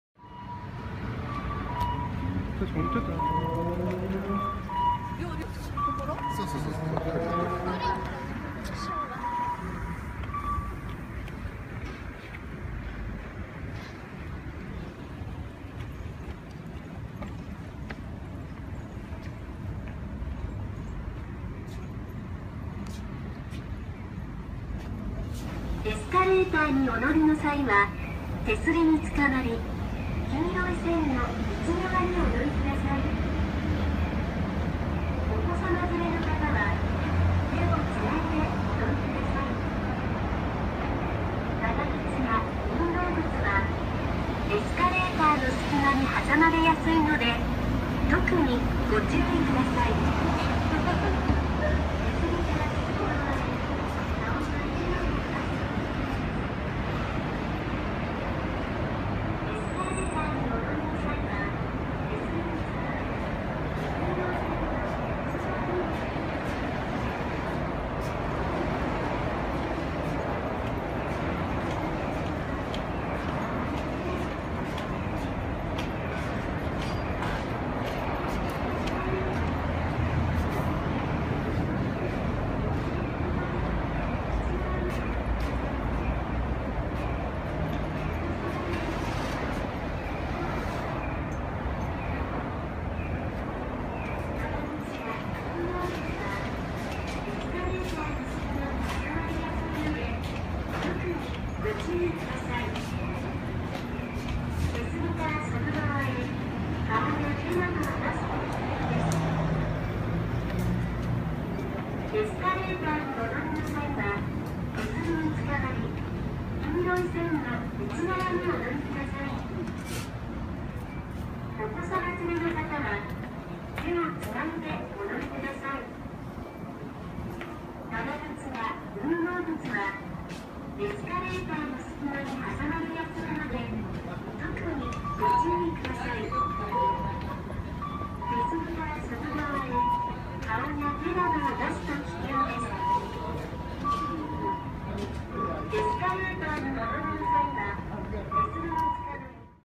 2014/11/22 イオンモール岡山歩道橋を歩いてみた
iPhone6 「Hyperlapse from Instagram」使用